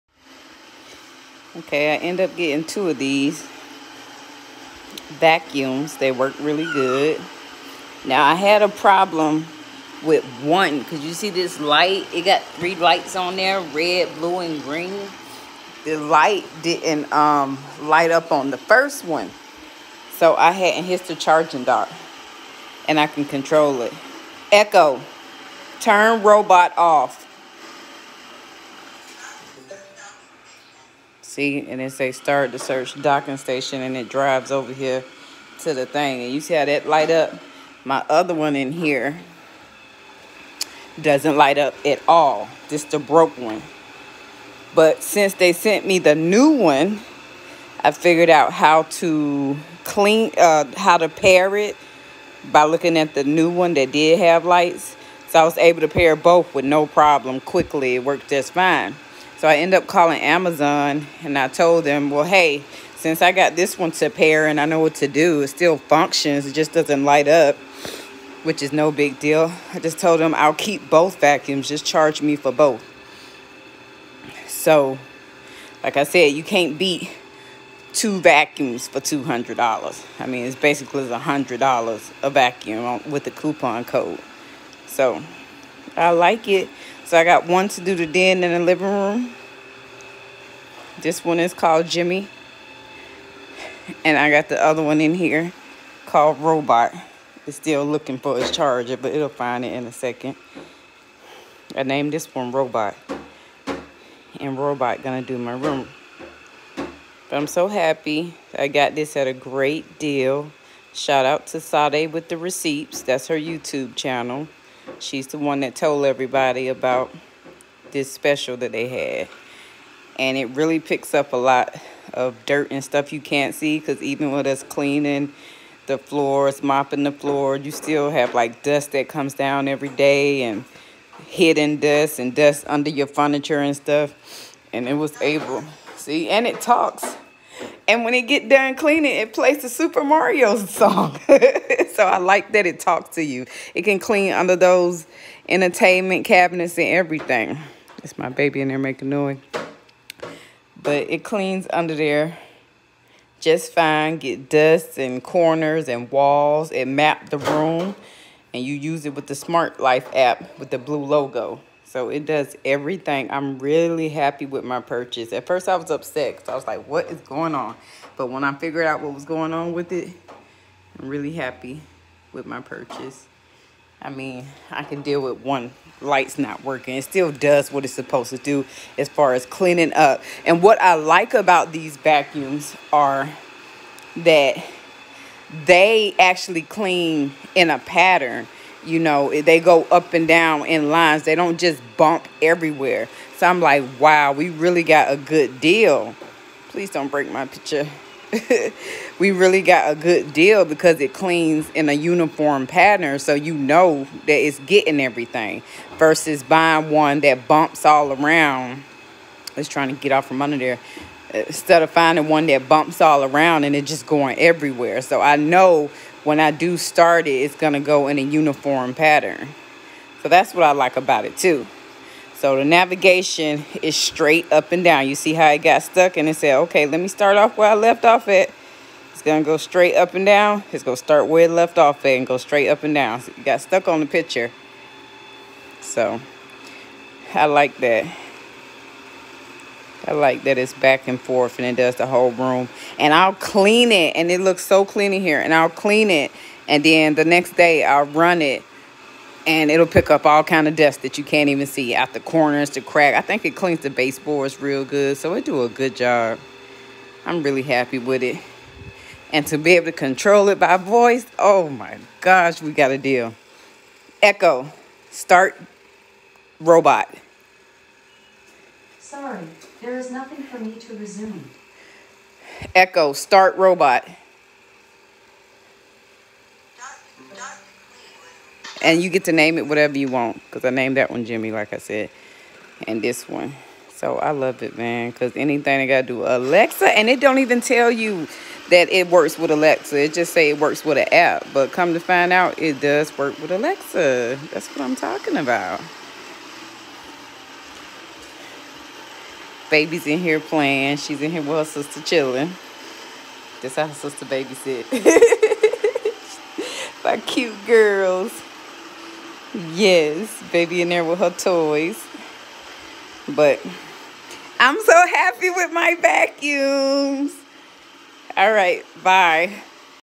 Aqwhz Robot Vacuums Amazon
Robot vacuum with Alexa and map smart navigation got on sale for $100 1800pa suction smart navigation talking vacuum lets you know when it’s stuck or have a error